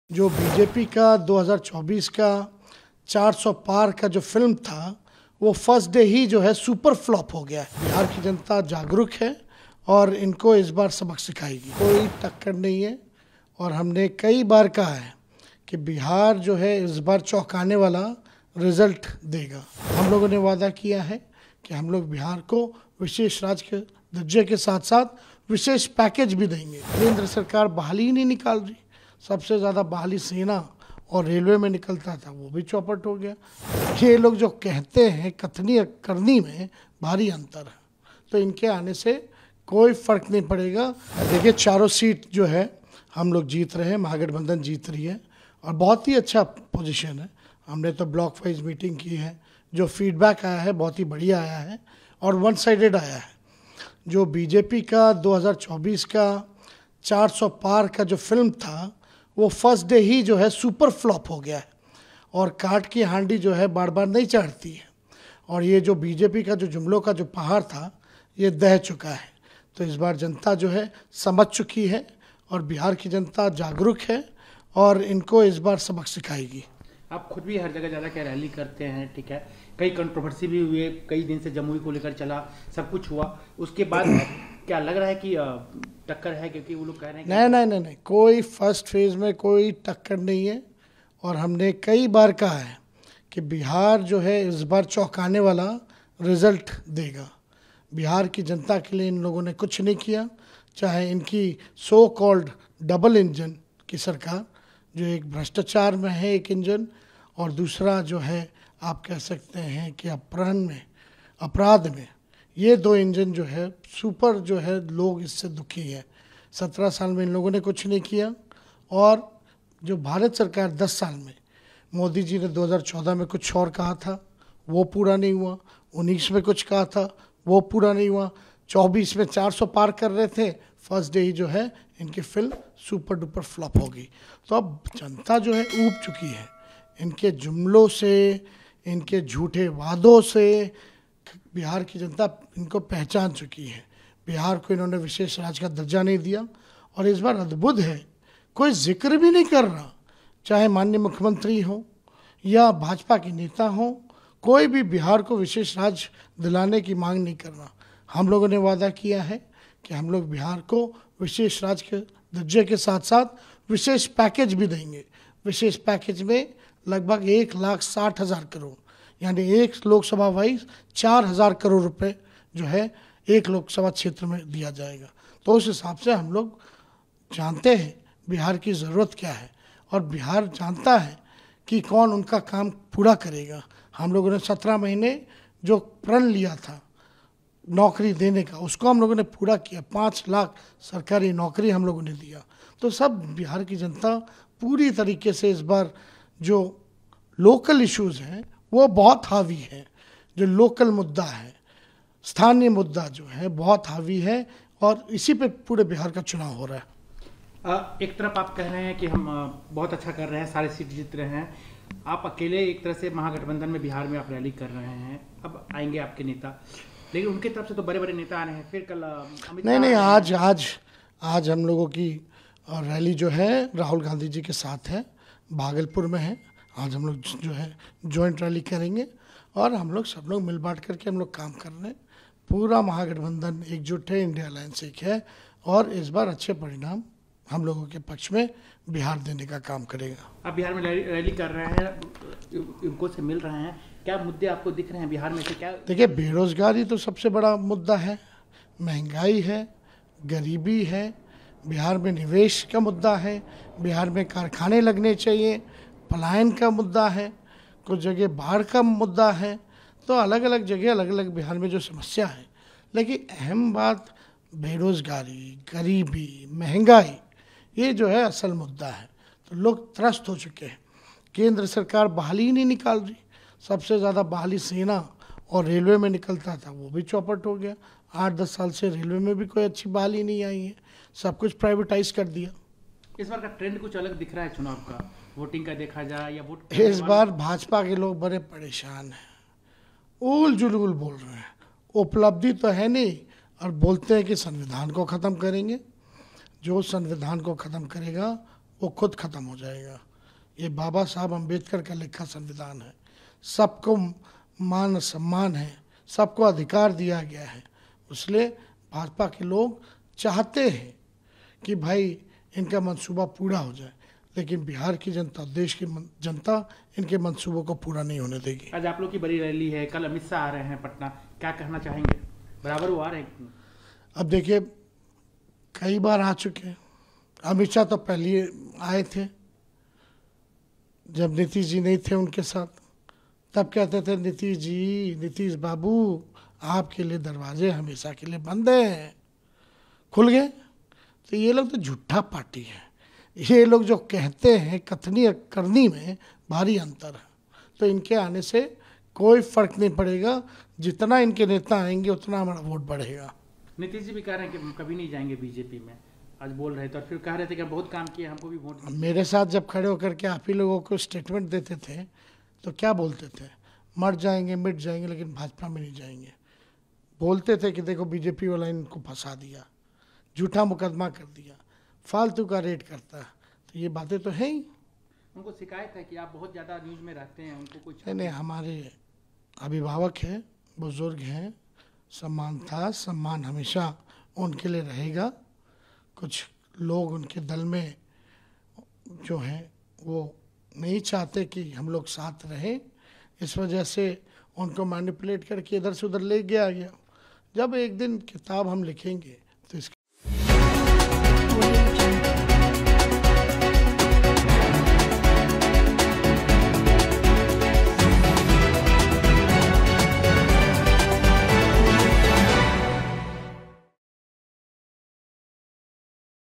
लोकसभा चुनाव: राजद के तेजस्वी यादव का कहना है कि बीजेपी की '400 पार' फिल्म पहले चरण में फ्लॉप हो गई
लोकसभा चुनाव: राजद के तेजस्वी यादव का कहना है कि बीजेपी की '400 पार' फिल्म पहले चरण में फ्लॉप हो गई है

After the completion of the first phase of the Lok Sabha Elections, Rashtriya Janata Dal (RJD) leader Tejashwi Yadav on April 20 took a dig at the Bharatiya Janata Party’s 400-paar slogan and said that the film has become a super flop on the first day itself.
"The Mahagathbandhan is winning all four seats in the first phase. We have held block-wise meetings and the feedback that has come is very good the BJP's "400 paar" film has become a super flop on the first day itself. The people of Bihar are aware and will teach them a lesson," he said